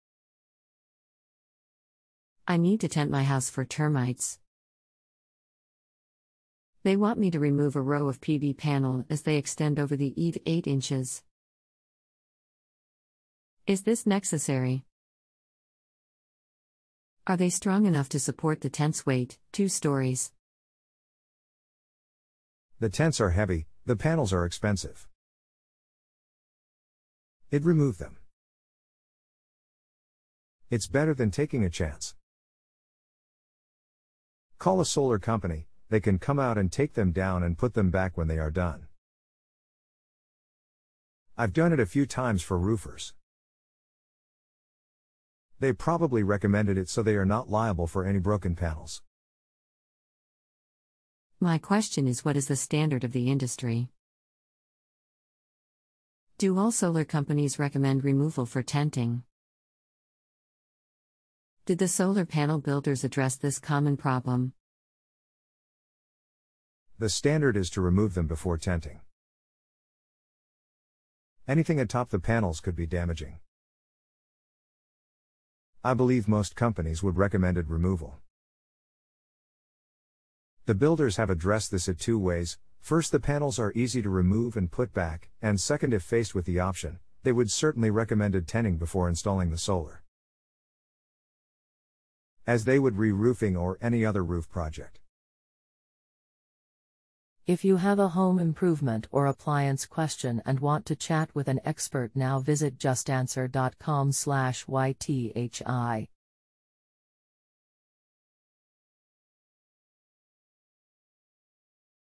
I need to tent my house for termites. they want me to remove
JustAnswer Customer: I need to tent my house for termites.
JustAnswer Customer: they want me to remove a row of PV panel as they extend over the eve 8".
JustAnswer Customer: Is this neccessary?
JustAnswer Customer: Are they strong enough to support the tents weight, 2 stories?
Douglas Feldt: : The tents are heavy, the panels are expensive
Douglas Feldt: : Id remove them.
Douglas Feldt: : Its better than taking a chance
Douglas Feldt: : call a solar company, they can come out and take them down and put them back when they are done
Douglas Feldt: : Ive done it a few times for roofers
Douglas Feldt: : They probably recommended it so they are not liable for any broken panels
JustAnswer Customer: My question is what is the standard of the industry?
JustAnswer Customer: Do all solar companies recommend removal for tenting?
JustAnswer Customer: Did the solar panel builders address this common problem?
Douglas Feldt: : The standard is to remove them before tenting.
Douglas Feldt: : Anything atop the panels could be damaging.
Douglas Feldt: : I believe most companies would recommended removal.
Douglas Feldt: : The builders have addressed this it two ways, first the panels are easy to remove and put back, and second if faced with the option, they would certainly recommended tenting before installing the solar.
Douglas Feldt: : As they would re-roofing or any other roof project.